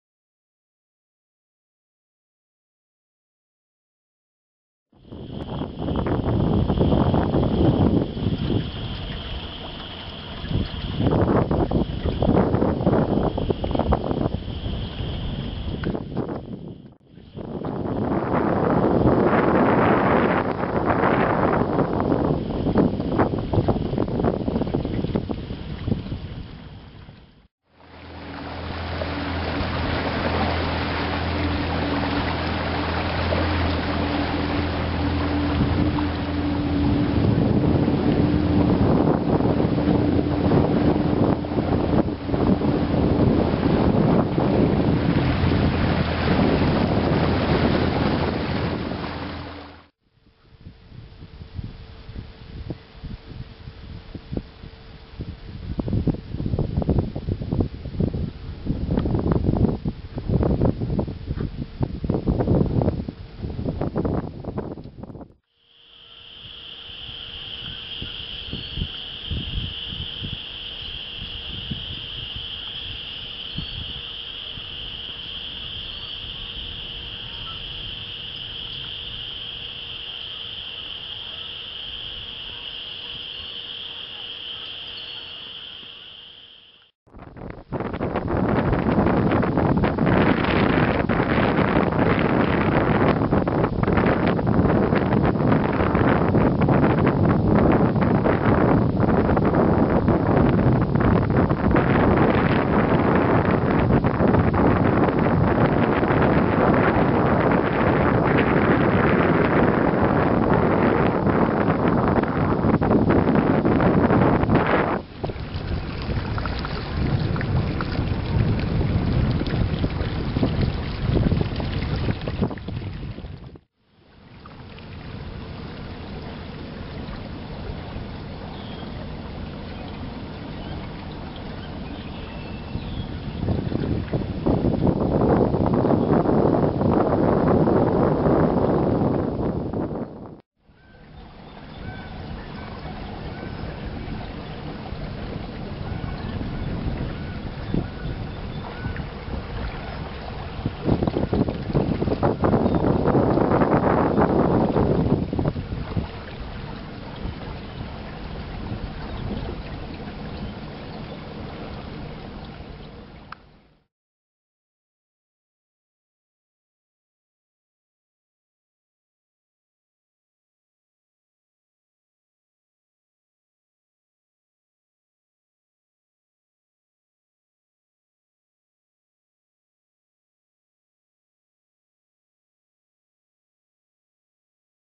Shiawassee Flats Recon 2011- Part 1 .wmv
A boat tour through the Shiawassee National Wildlife Refuge, what I call the "wide part". Exploring a majority of the tributaries to the Great Lakes Bay Region. Only minutes from Frankenmuth, Saginaw, Bay City, and Birch Run.